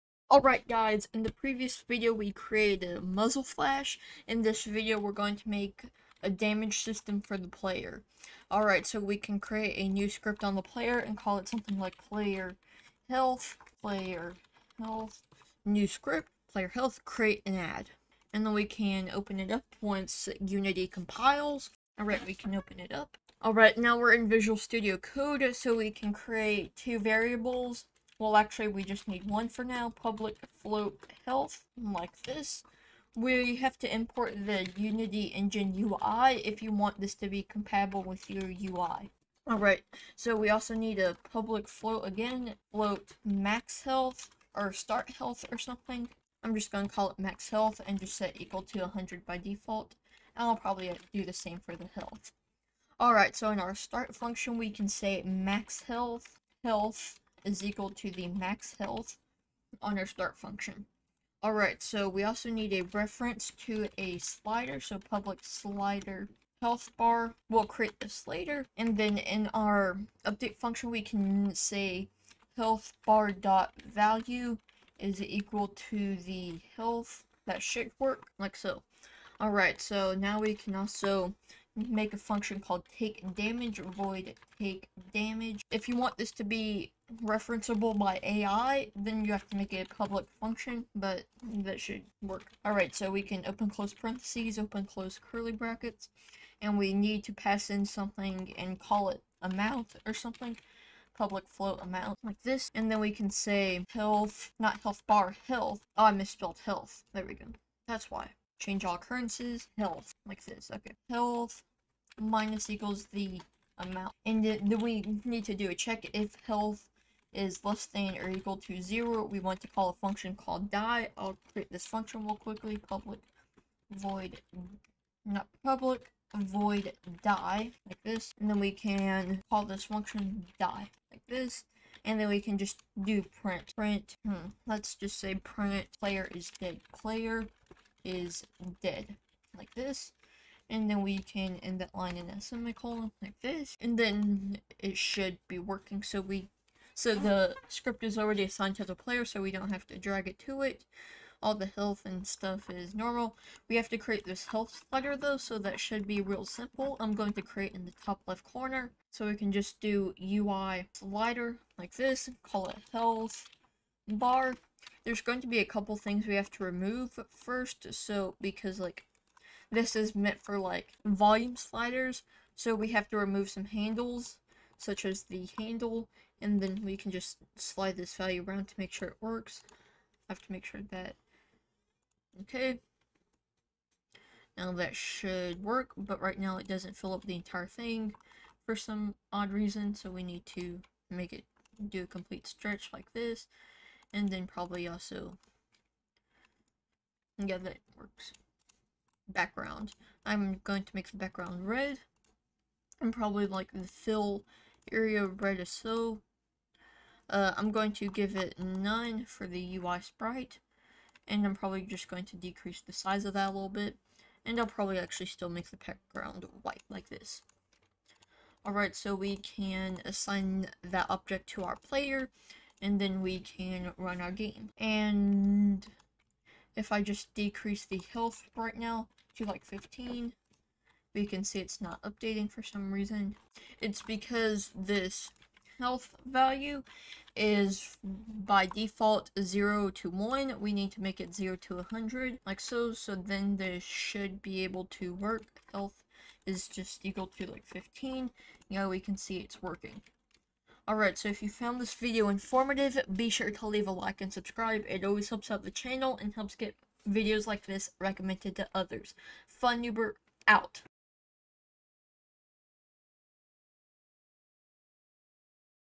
Create A Health System in Unity! | Unity First Person Shooter Series
If you have ideas for videos then please commet the idea below! I am always looking for new ideas so I can educate you.
In this video I will show you how you can make a player health system for your unity first person shooter game!